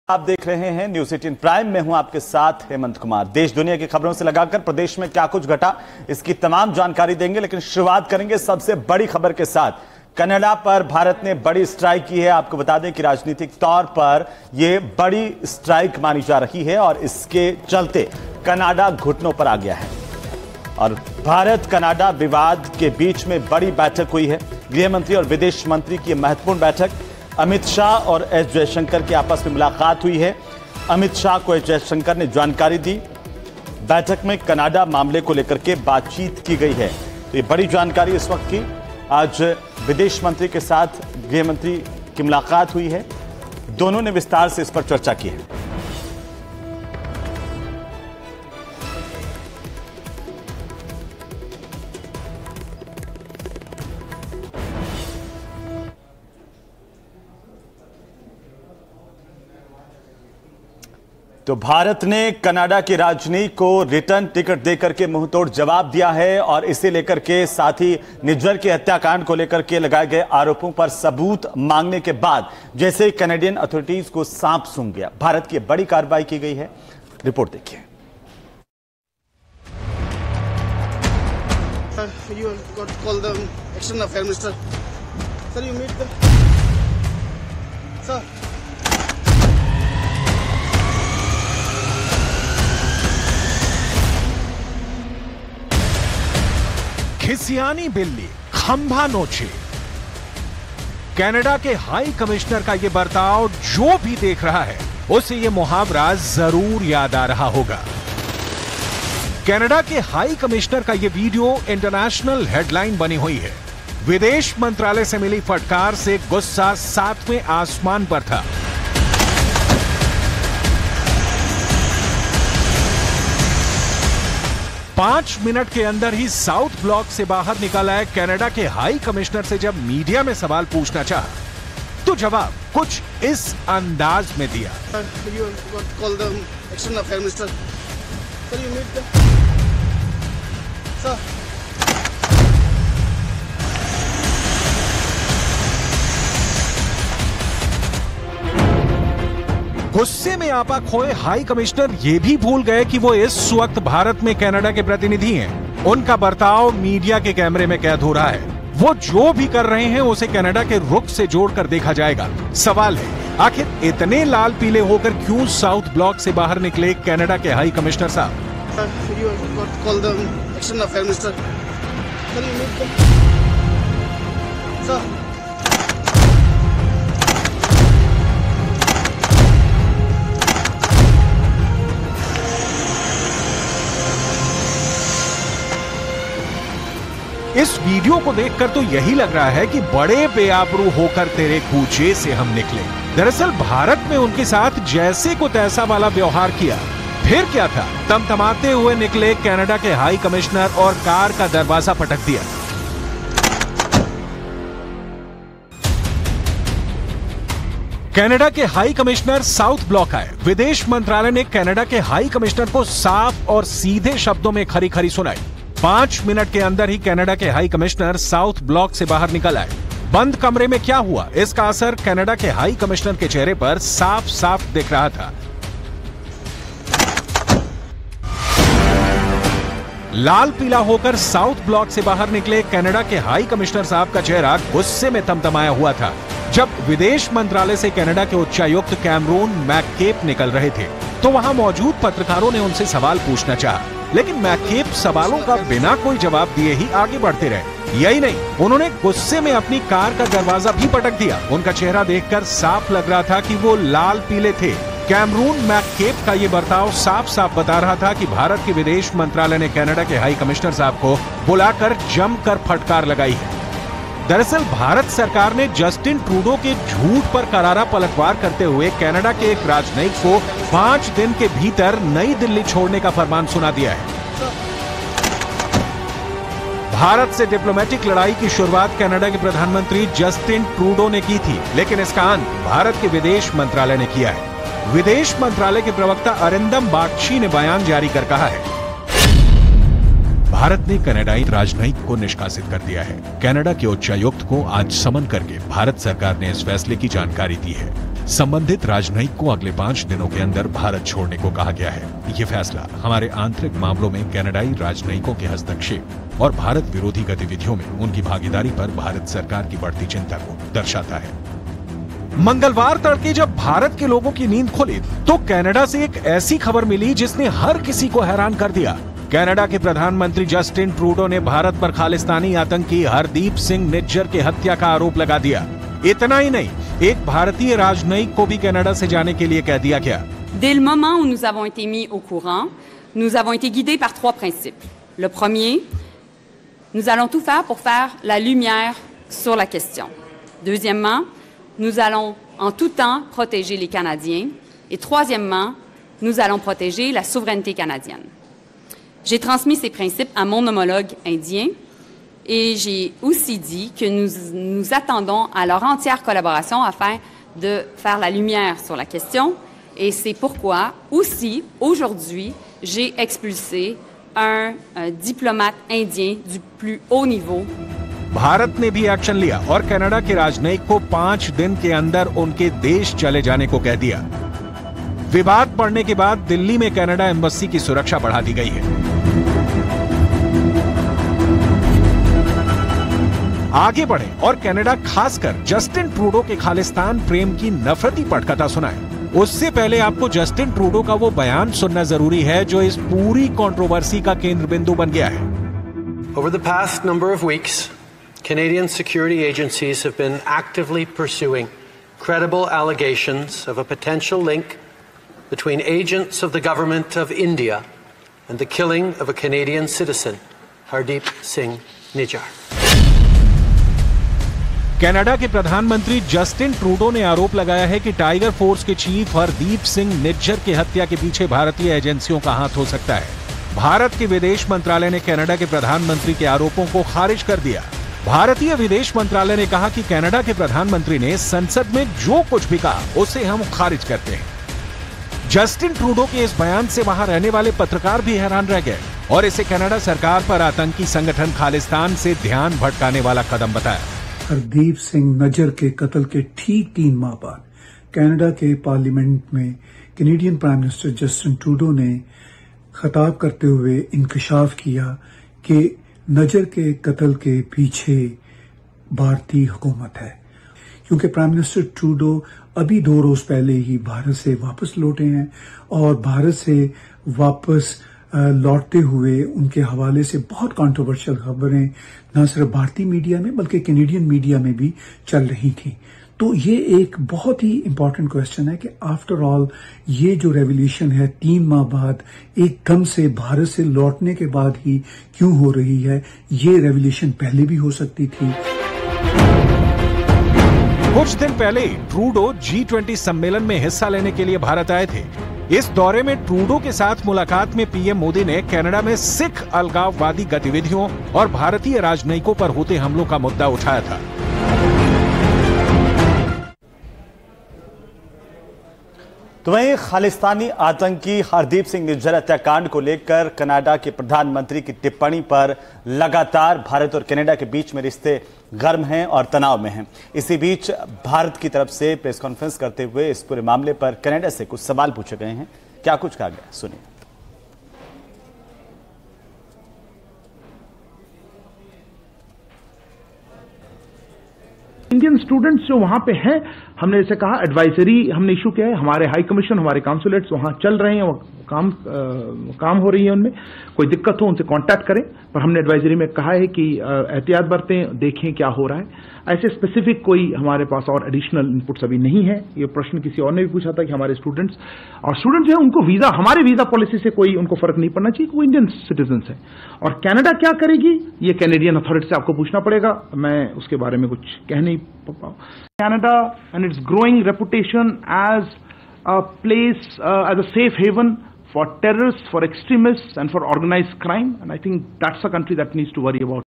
India- Canada Relations News : भारत ने कनाडा के आरोपों पर दिखाई सख्ती, Amit Shah ने बुलाई बैठक
Khalistani Hardeep Singh के Murder पर Canada और India में Controversy तेज होती जा रही है. Canada पर अब India बड़ा Action ले सकता है.